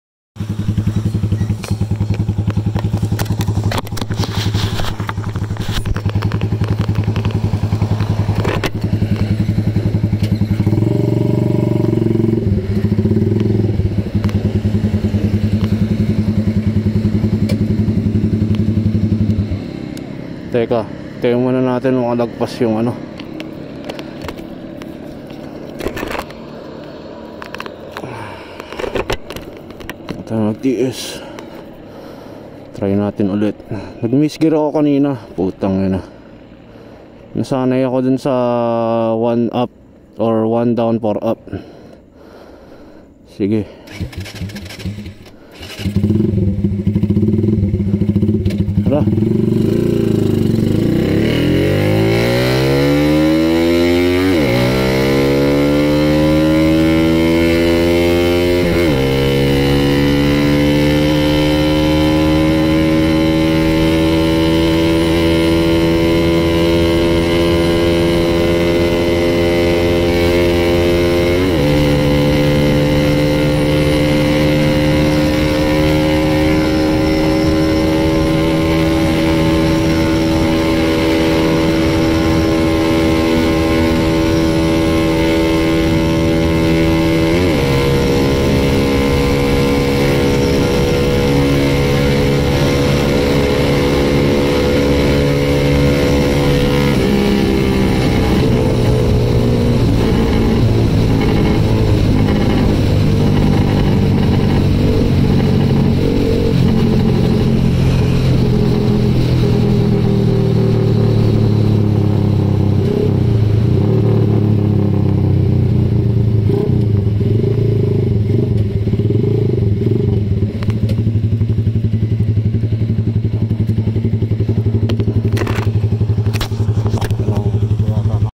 Rusi SSV200 Gazelle part 3
3rd attempt trying to top speed my rusi ssv200 gazelle. KULANG SA RUNWAY!!